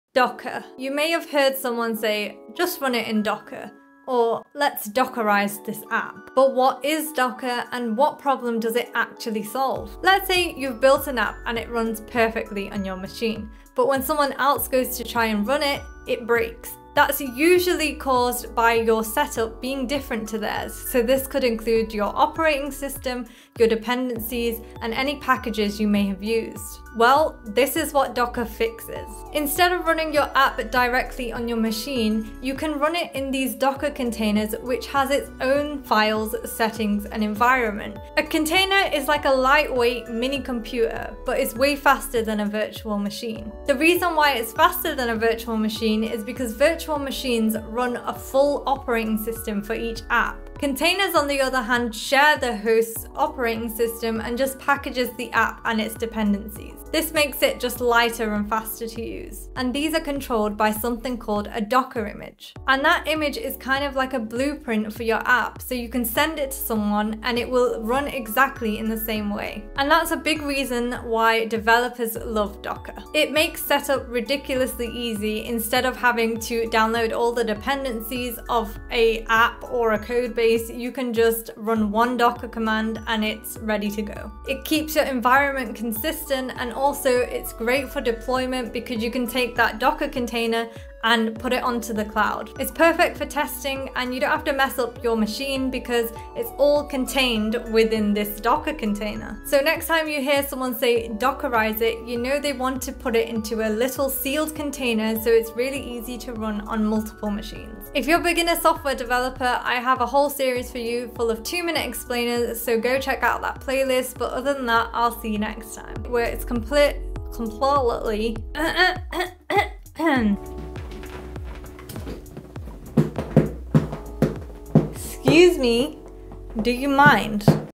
What is Docker? | Explained in 2 Minutes
In this video, I break down what Docker is — in plain English. We'll look at how it works, why it's different from virtual machines, and how it helps developers run apps consistently across different environments.